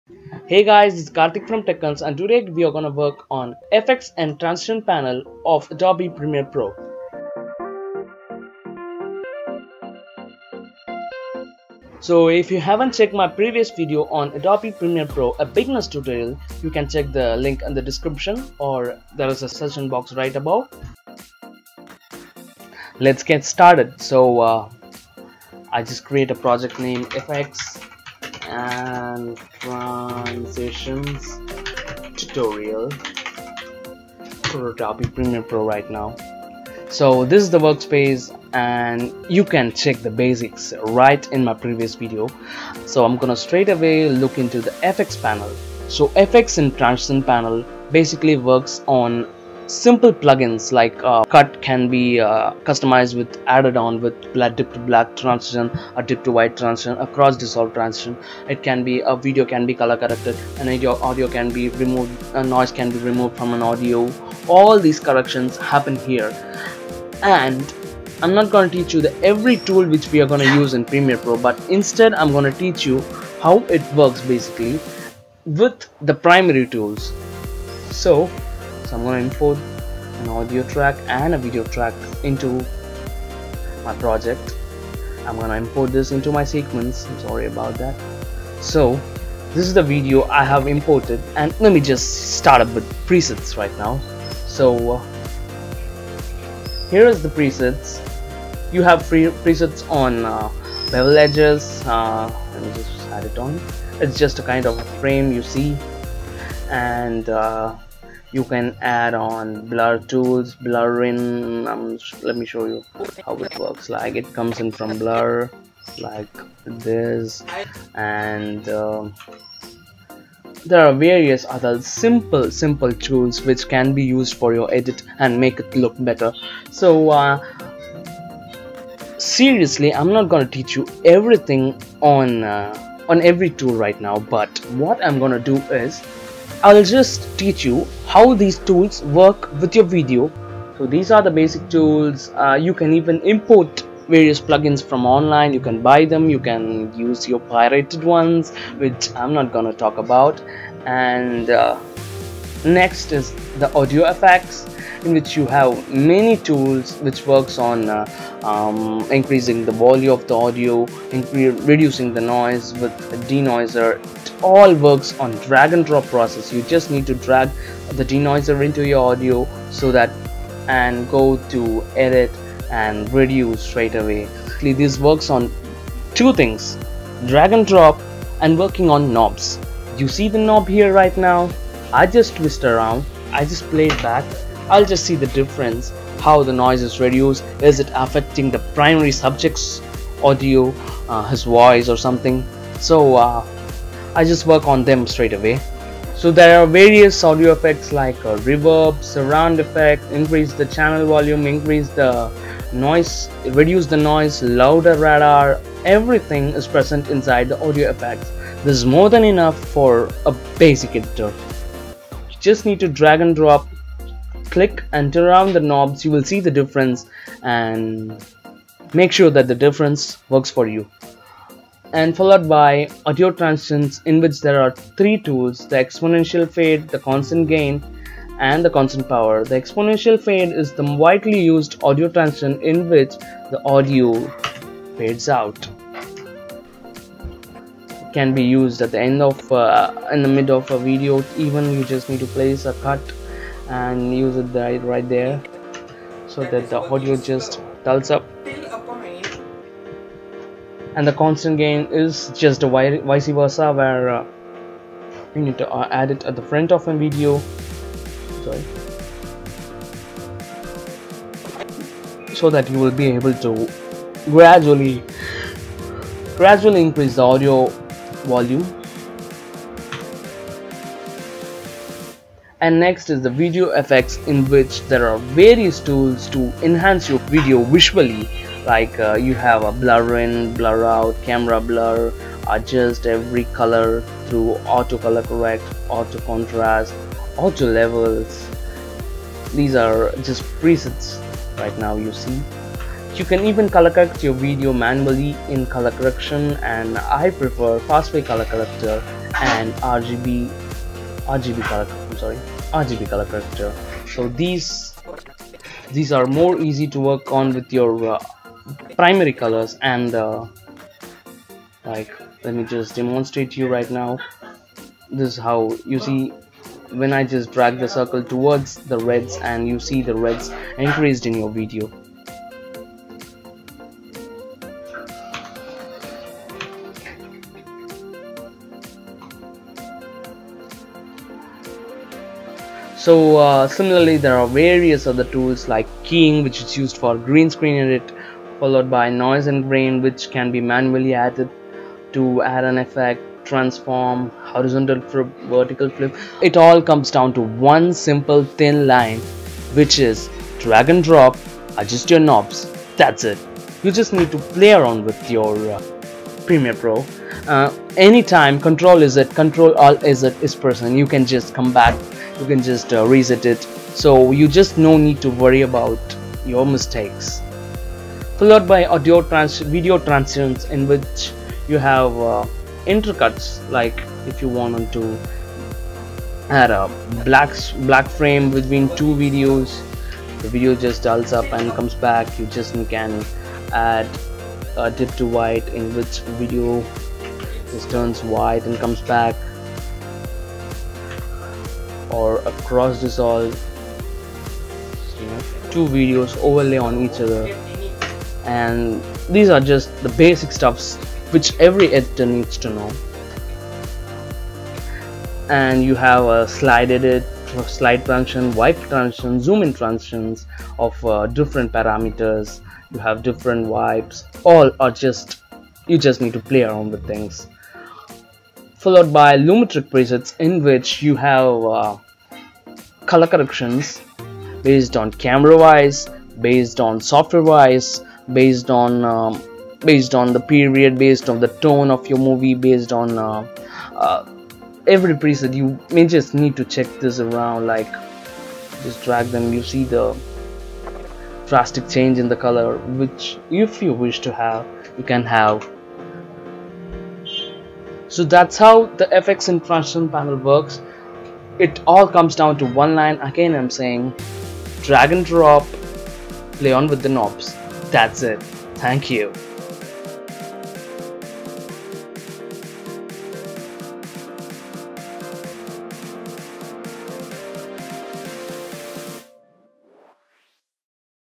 EFFECTS AND TRANSITIONS | ADOBE PREMIERE PRO CC/CS6 TUTORIAL
A Beginner Tutorial on how to work on effects and transitions panel in adobe premiere pro.
If you haven't checked out my previous video on how to work on Adobe Premiere pro | Beginners Tutorial
A Tech Hunts Presentation.